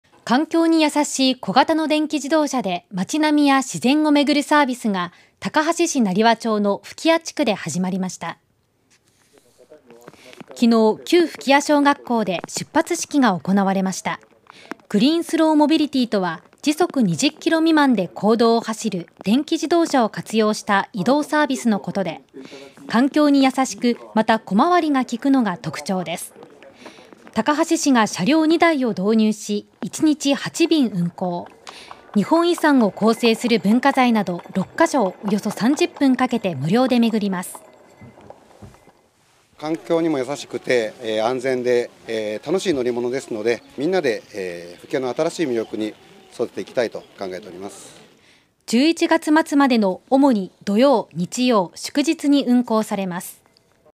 環境にやさしい移動手段で日本遺産を 高梁市成羽町吹屋地区でグリーンスローモビリティ運行開始【岡山】
環境にやさしい小型の電気自動車で街並みや自然を巡るサービスが高梁市成羽町の吹屋地区で始まりました。4月29日、旧吹屋小学校で出発式が行われました。グリーンスローモビリティとは、時速20キロ未満で公道を走る電気自動車を活用した移動サービスのことで環境にやさしく、また小回りが効くのが特徴です。（2023年4月29日放送）